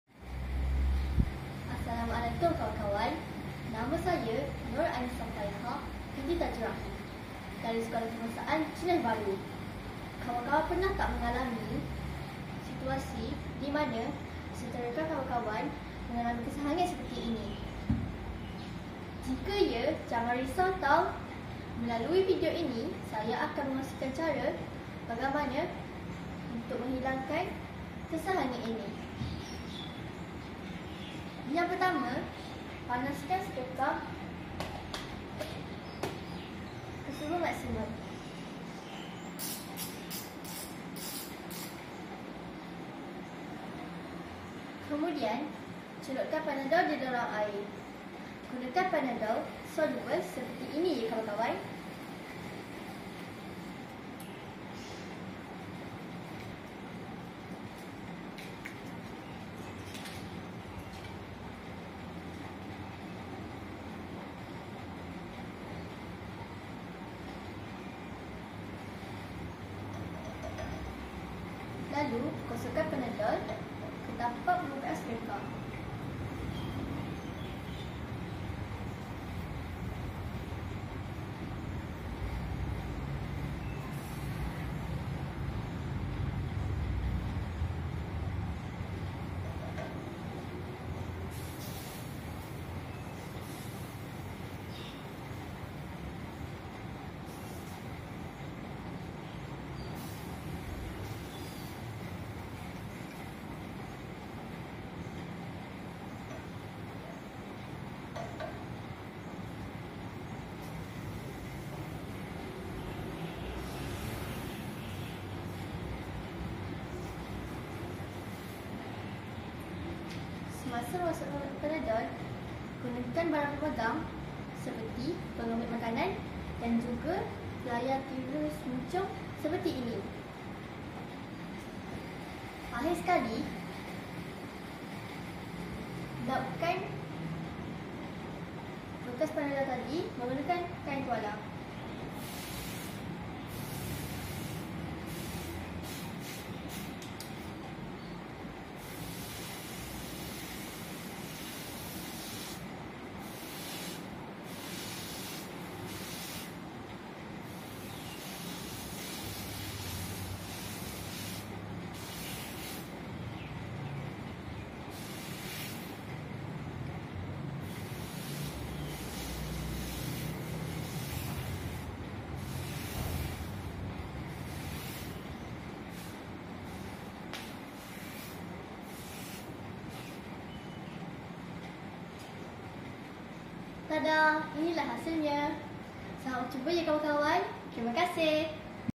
Pertandingan Menghilangkan Tanda Karat/Kotor pada Seterika
Pertandingan Sempena Hari Puteri Peringkat Kebangsaan PPIM Tahun 2023 oleh Nur Annisa Fayhaa Binti Tajur Rahim